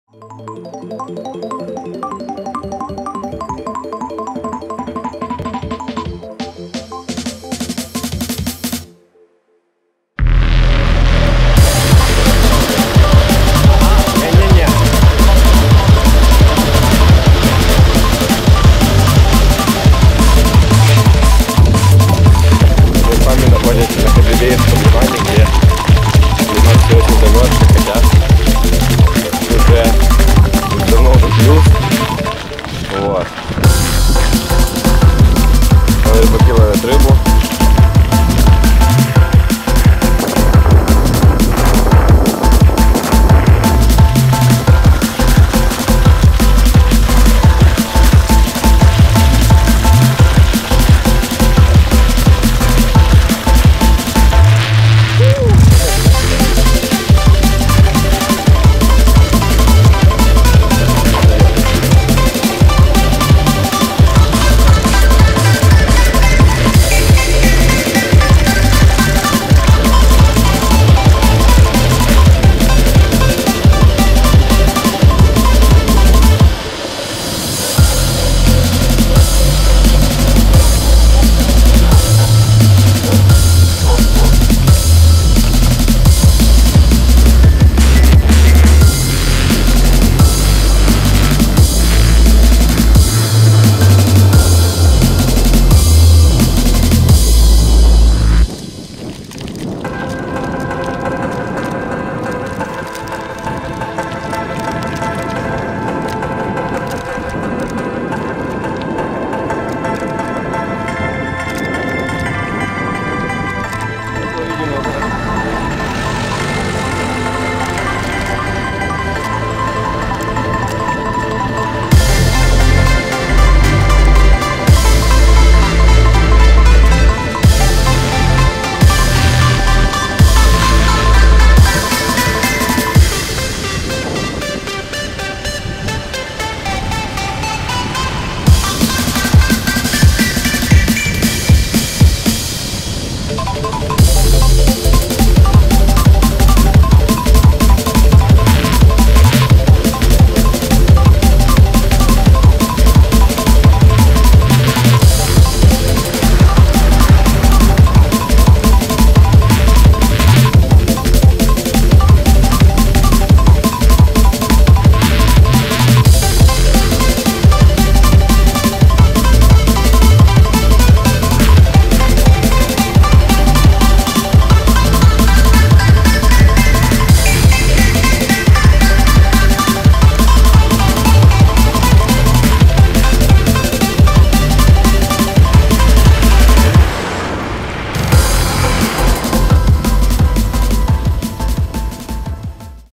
Spring cycling [Odessa]
подготовка к велосотке 2012
40 км. воскресным утром [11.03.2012]